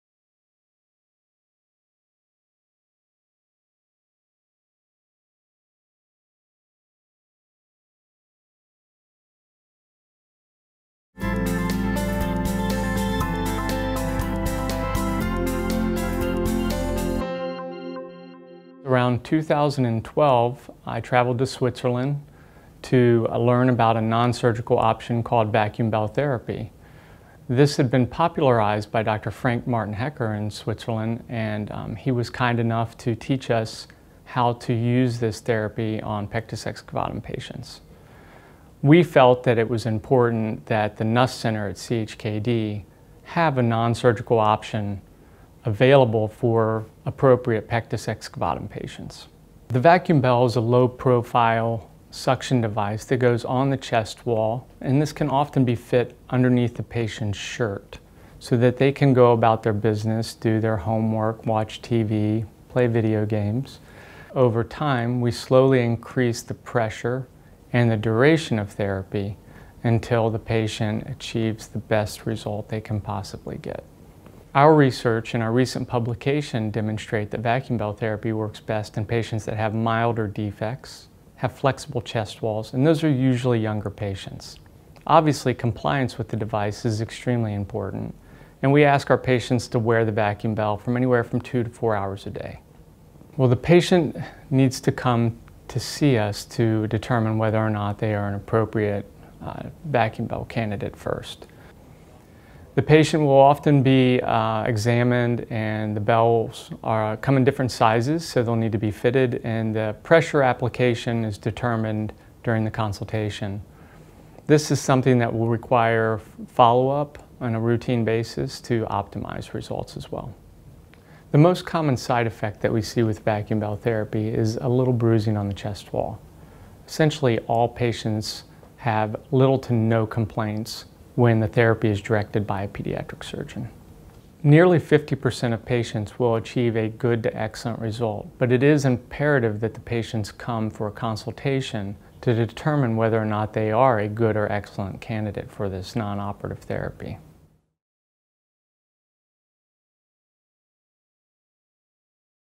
Vacuum Bell - Non Surgical Correction of Pectus Excavatum
Robert Obermeyer, MD, discusses non-surgical options to correct pectus excavatum and pectus carinatum.